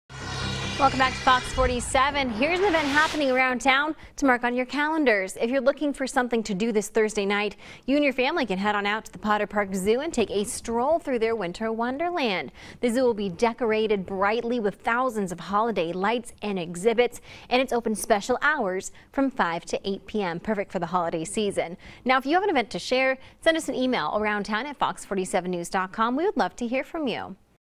Potter Park Zoo decorated with thousands of lights
Take a stroll through Potter Park Zoo's winter wonderland. It's decorated with thousands of holiday lights and exhibits and open special hours 5 p.m to 8 p.m.